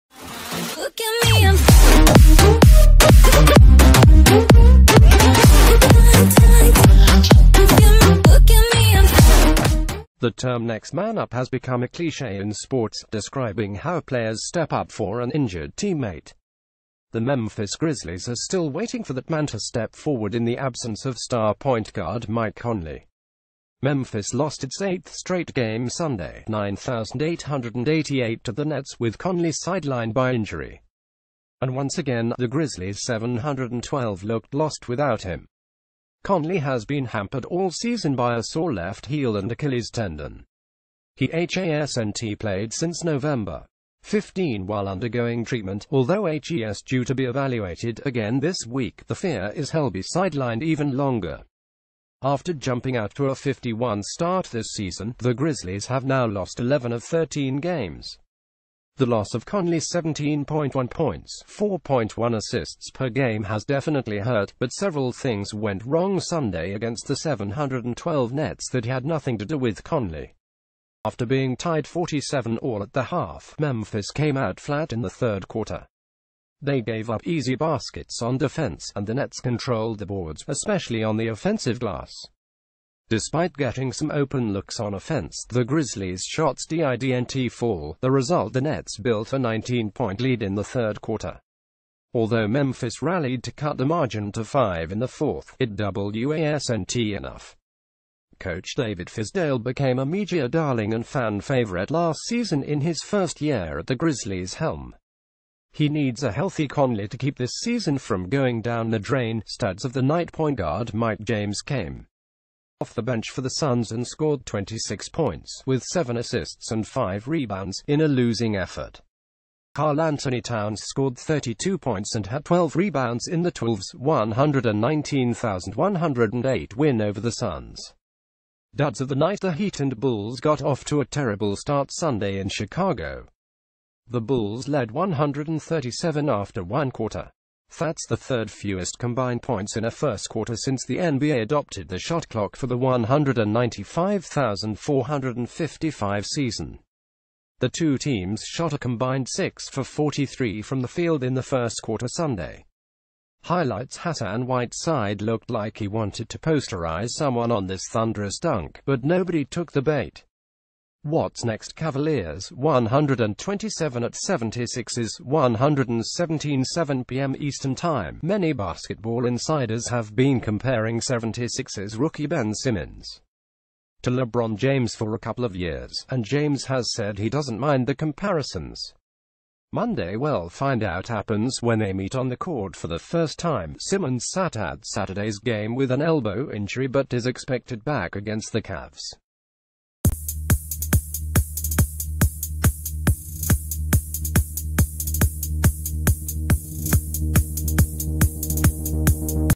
Nba wrap: grizzlies' losing streak reaches 8 games following loss to nets
The term next man up has become a cliche in sports, describing how players step up for an injured teammate. The Memphis Grizzlies are still waiting for that man to step forward in the absence of star ...